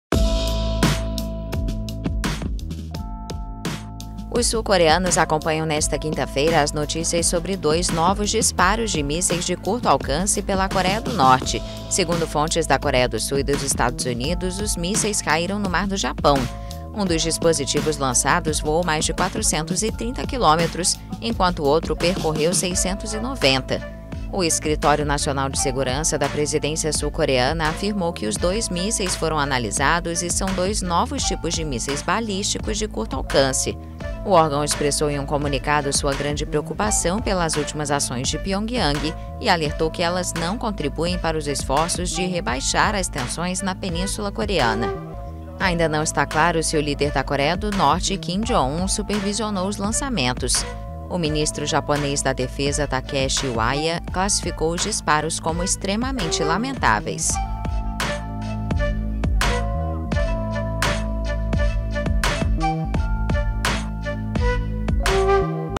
Coreia do Norte lança dois mísseis de curto alcance
Os sul-coreanos, acompanharam as notícias sobre os disparos feitos pelo país vizinho. Os dois projéteis pararam no mar do Japão.